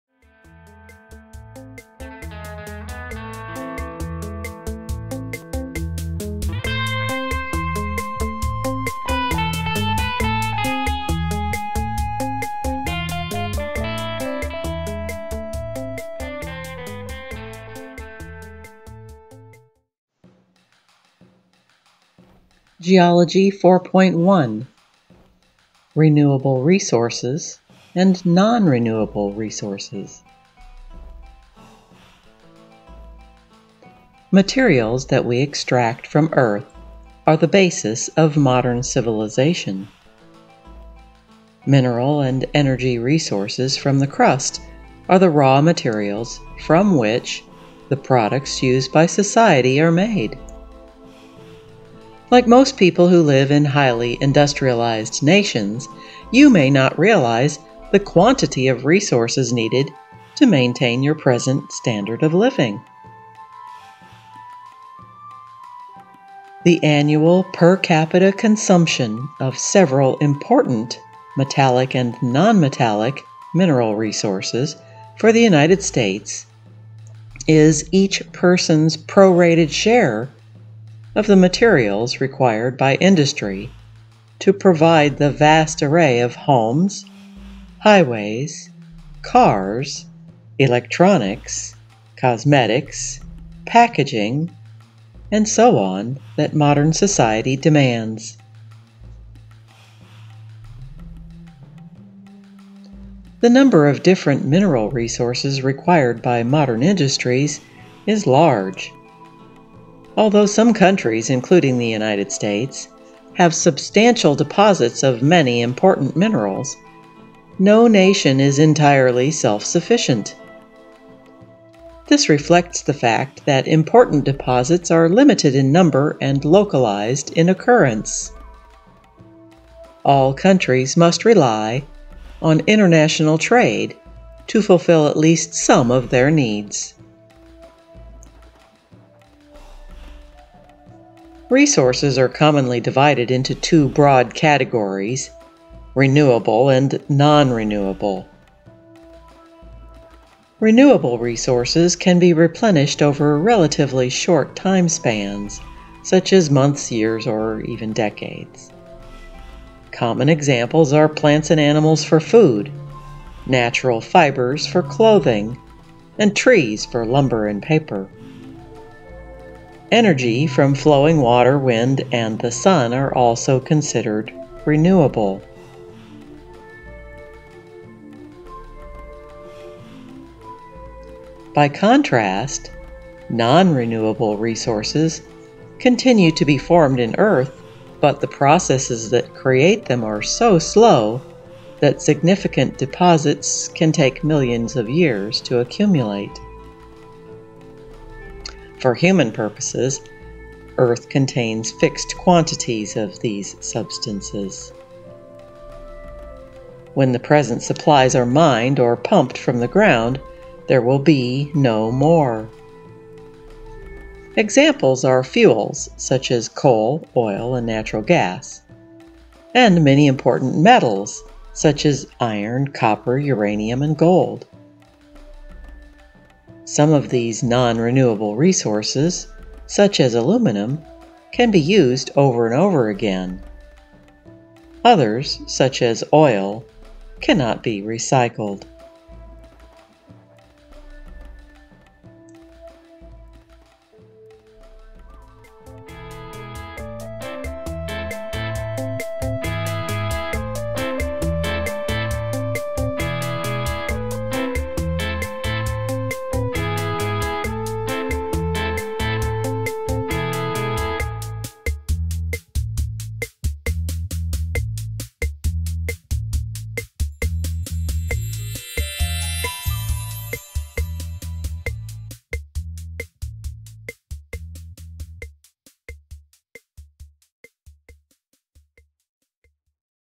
Geology 4.1: Renewable Resources and Non-Renewable Resources
See our playlist of videos for Geology & Earth Science

Materials that we extract from Earth are the basis of modern civilization. Mineral and energy resources from the crust are the raw materials from which the products used by society are made. Like most people who live in highly industrialized nations, you may not realize the quantity of resources needed to maintain your present standard of living.

The annual per capita consumption of several important metallic and non-metallic mineral resources for the United States is each person's prorated share of the materials required by industry to provide the vast array of homes, highways, cars, electronics, cosmetics, packaging, and so on that modern society demands.